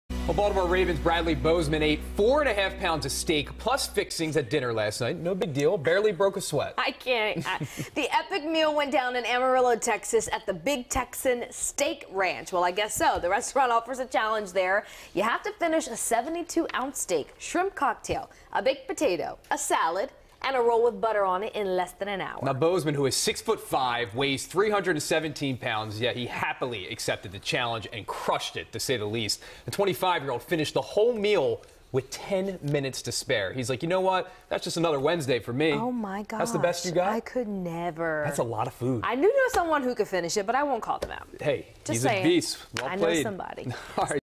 Ravens' Bradley Bozeman Wins Steak-Eating Challenge
Could you eat more than four pounds of steak in an hour? The Ravens' Bradley Bozeman can, and he did it with time to spare!